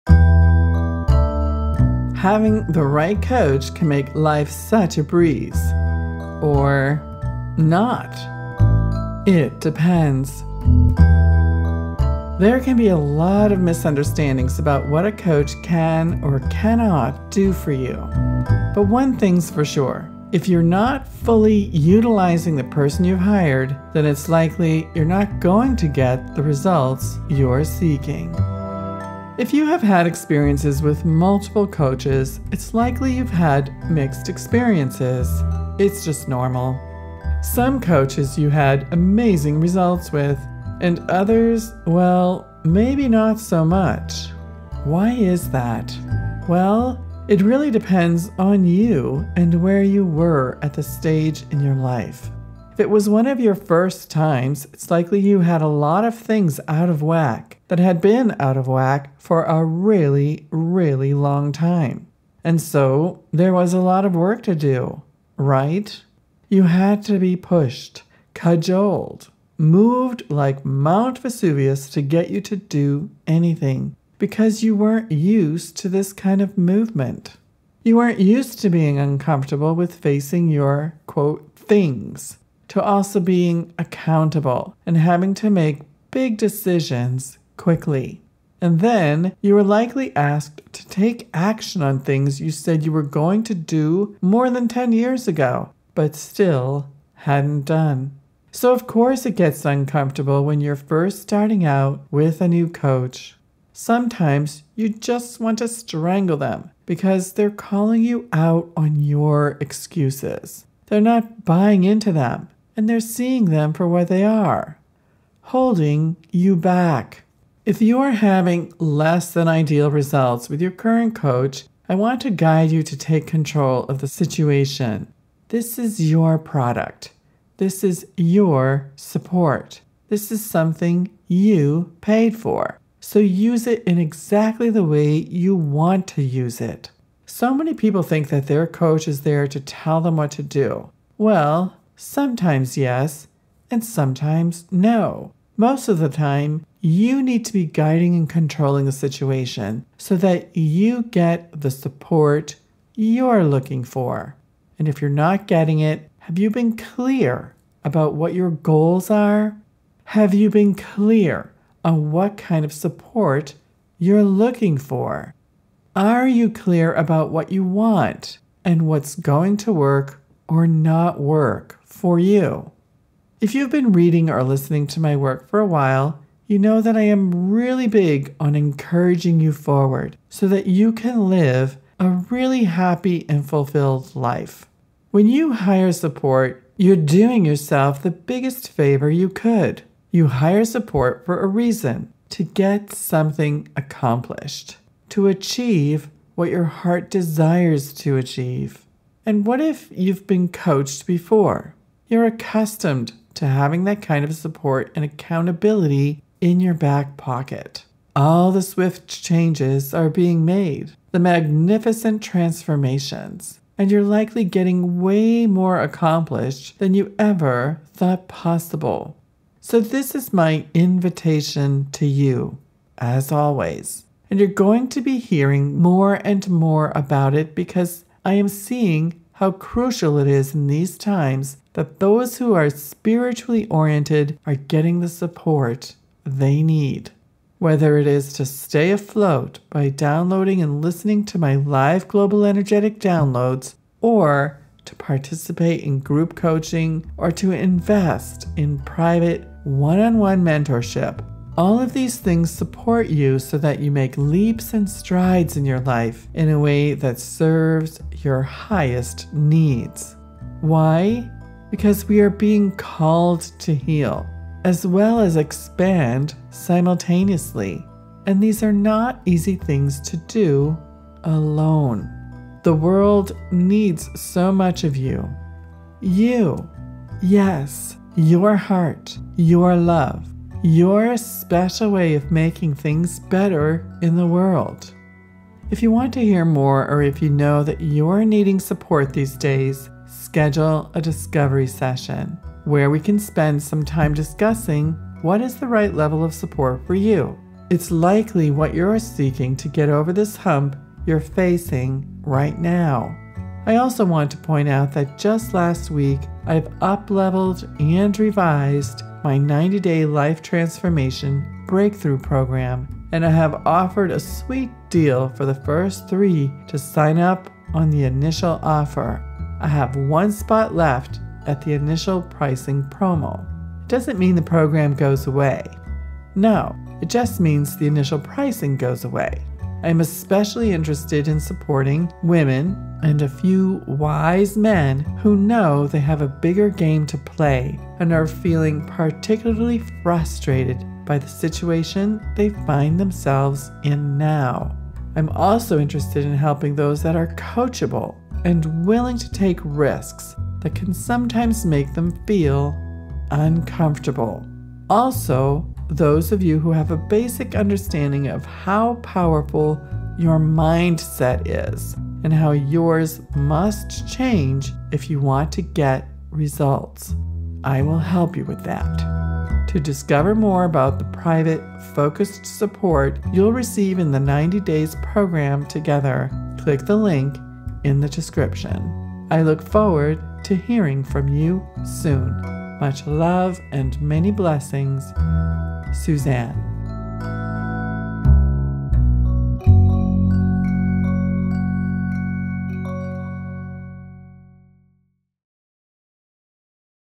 EP. 101.5 Having the right coach can make life such a breeze...
There can be a lot of misunderstandings about what a coach can or cannot do for you.  But one thing's for sure. If you're not fully utilizing the person you've hired, then it's likely you're not going to get the results you're seeking.  Listen here to learn more.

 Interested to know about the "90-Day Life Transformation Breakthrough" Program?